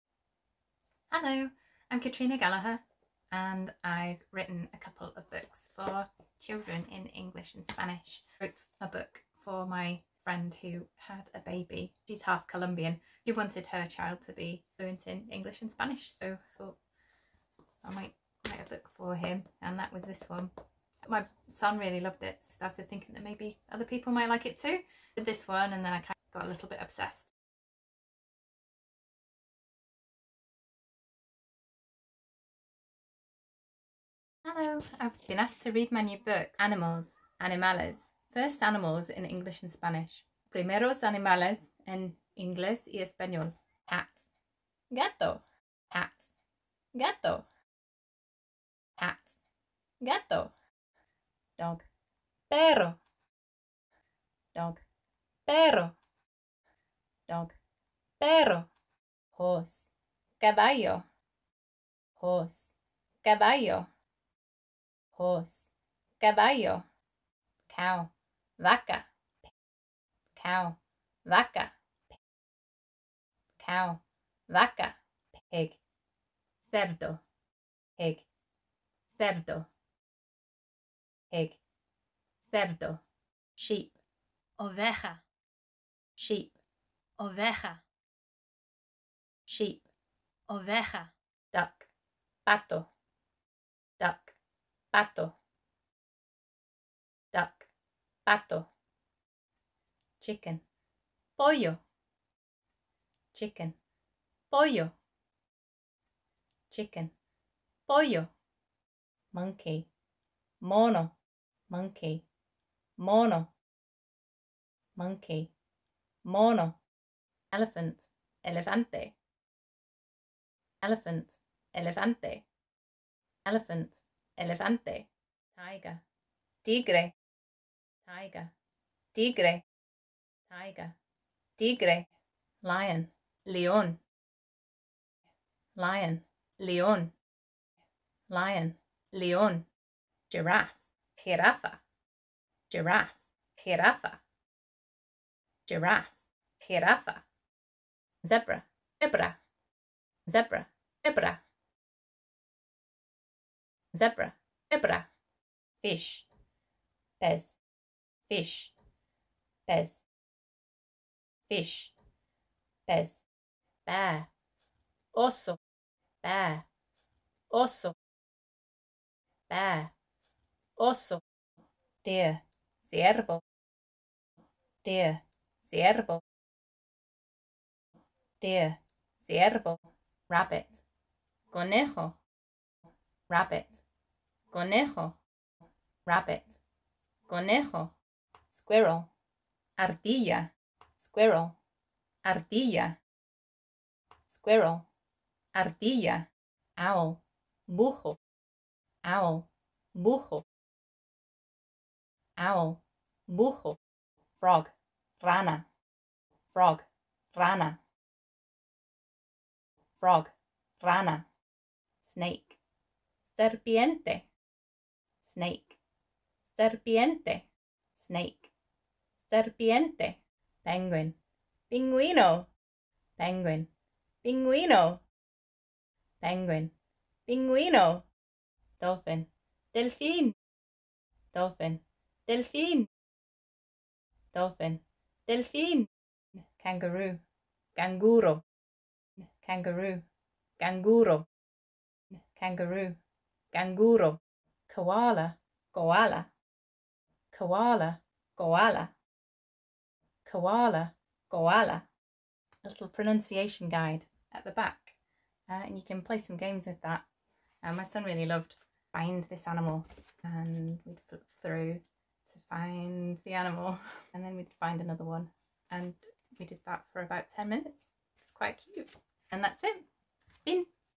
Animals/Animales: First animals in English and Spanish read aloud by author Katrina Gallagher
An ideal place to bring your animal-loving child’s linguistic journey!  Your child can listen and copy the words that are read out loud by the author Katrina Gallagher!

Pre-school, reception, KS1 and KS2 Spanish and English animal words.